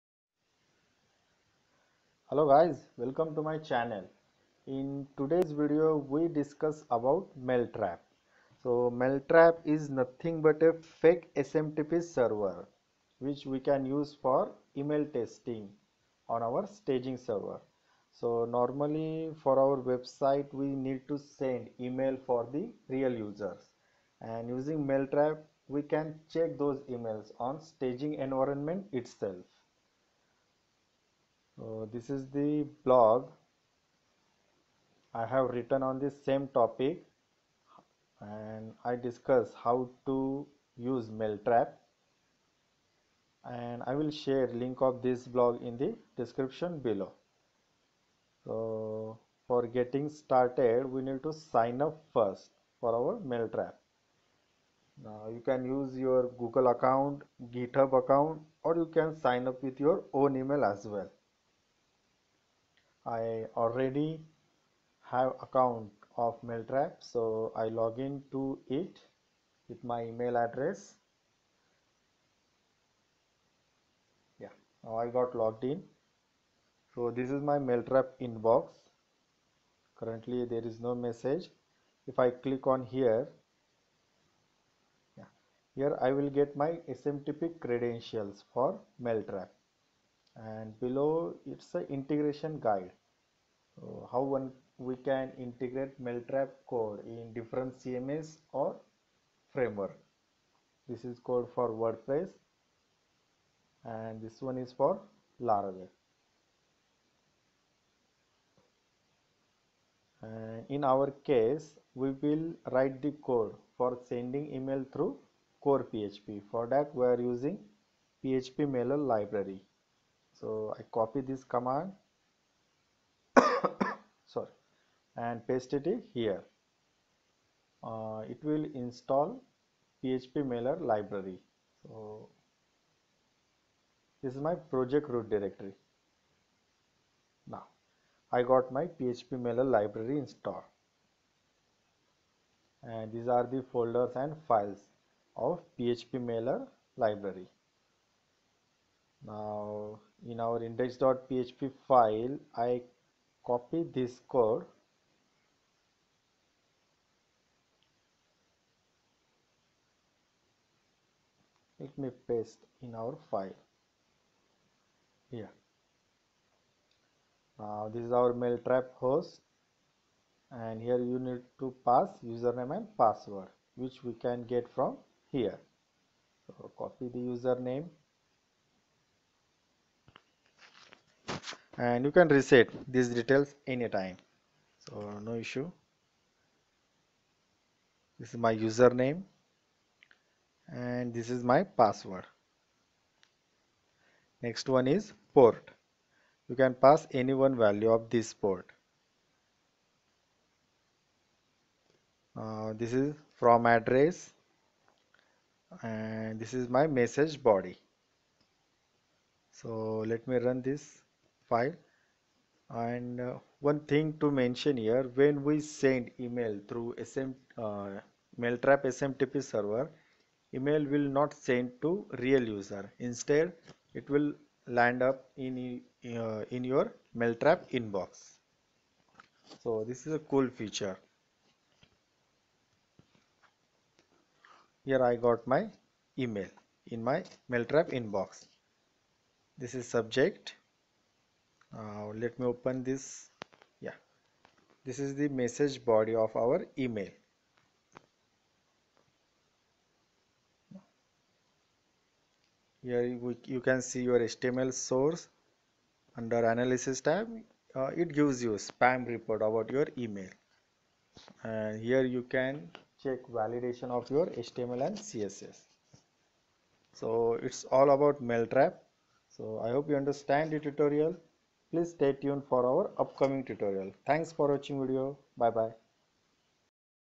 Mailtrap - A Fake SMTP Server For Pre-Production Email Testing
Emails are an important factor for any website. We used to send emails about user registration, newsletters, new deals or coupons, etc. Before these emails start sending to real users, we should do email testing in the development phase. In this video, we study how to do sandbox testing for your emails using Mailtrap.